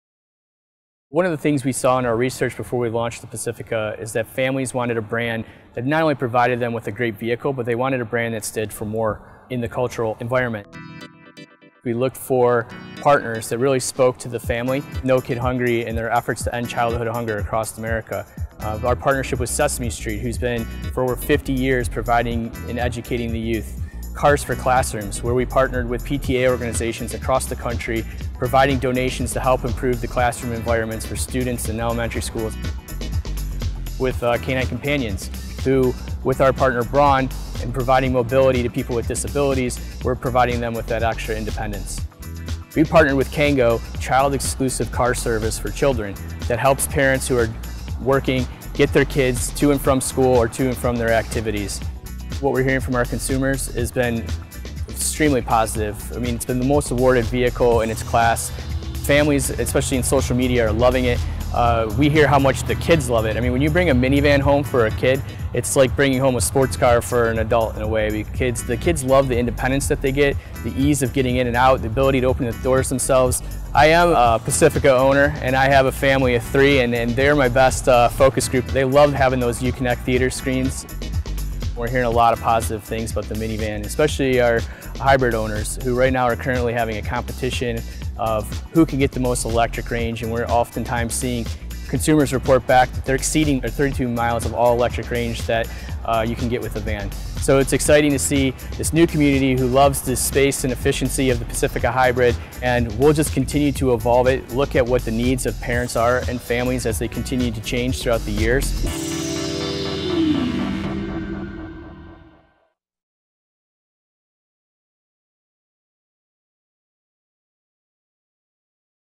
Kids & Pets Love Chrysler Pacifica! | Minivan Partnerships | NowCar.com
Vincent Maccani, head of marketing operations for the Chrysler brand, talks about how extensive research by the brand into the needs and desires of today's families for the Chrysler Pacifica and Pacifica Hybrid minivans led to a number of new partnerships that help positively affect the cultural environment.